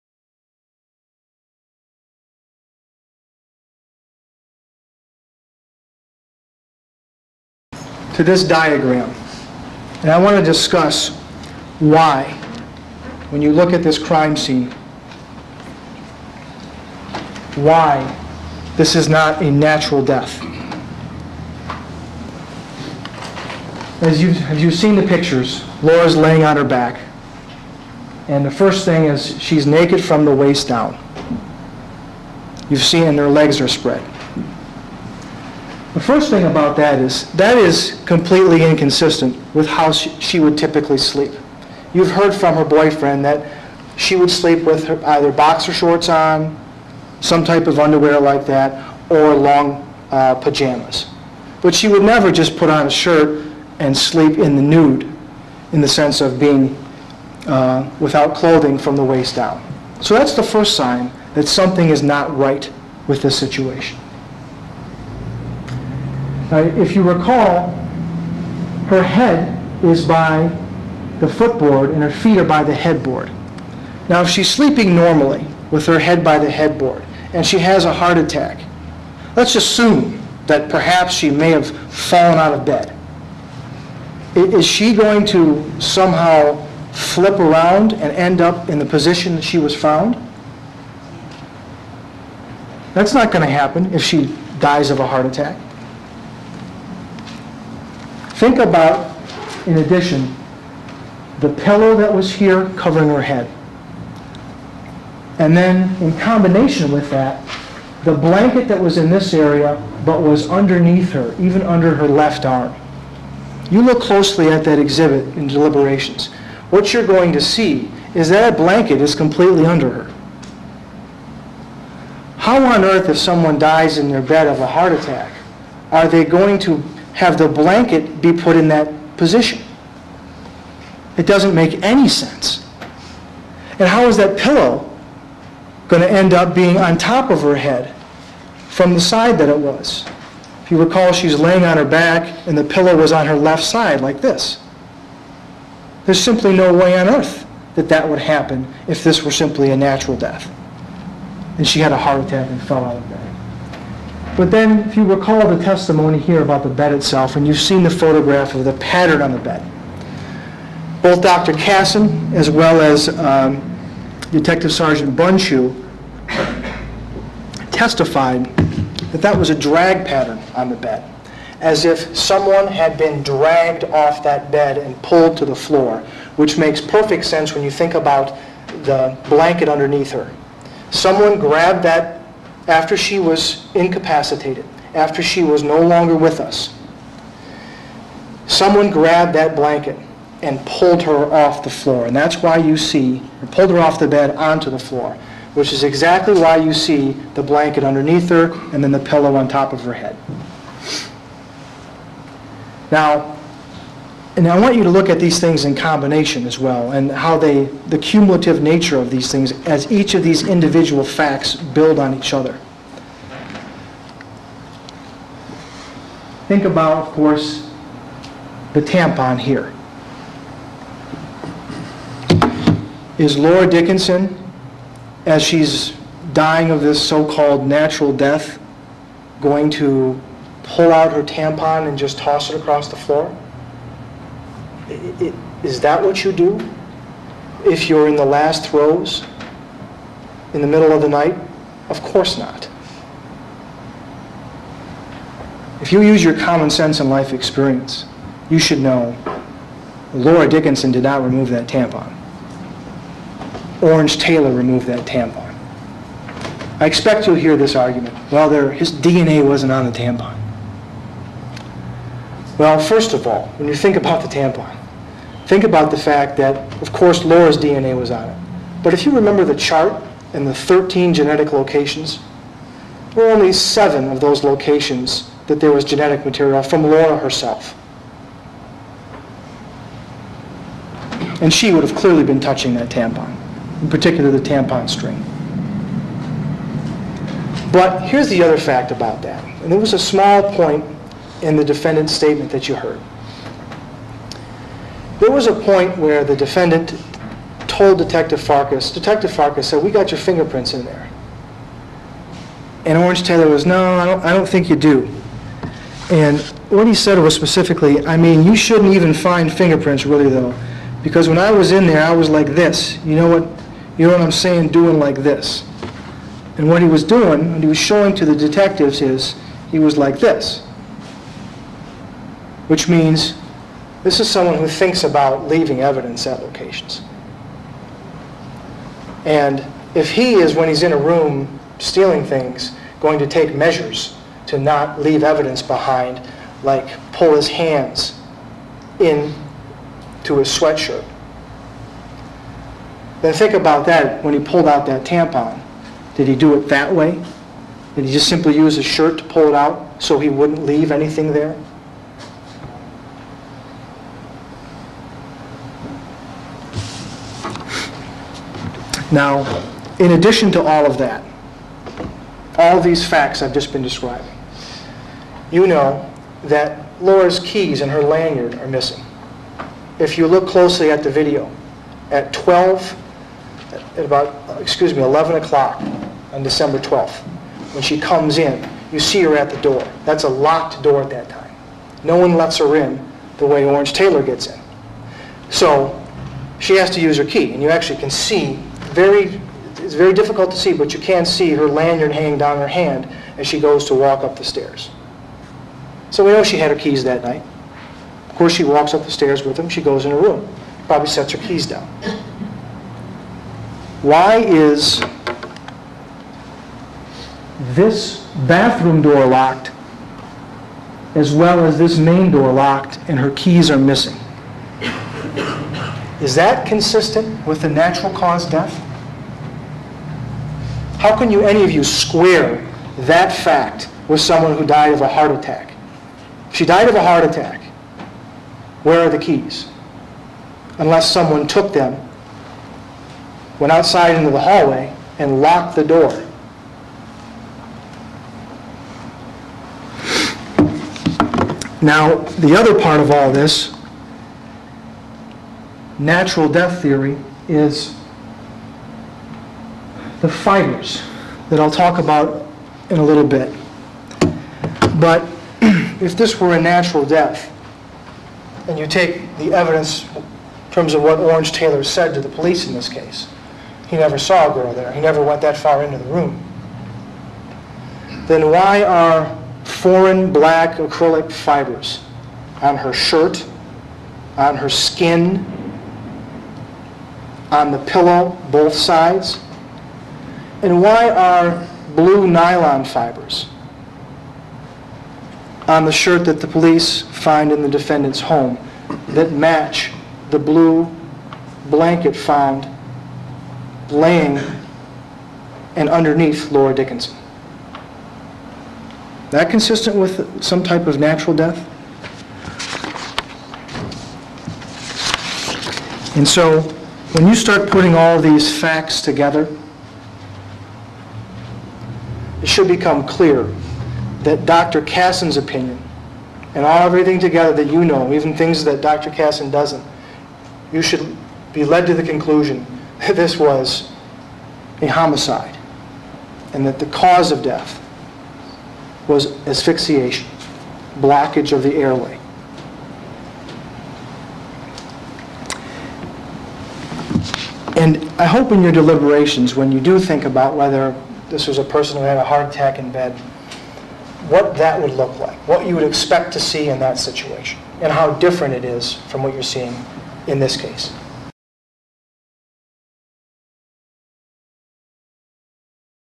Orange Taylor trial | The Ann Arbor News
Prosecution closing arguments part 2.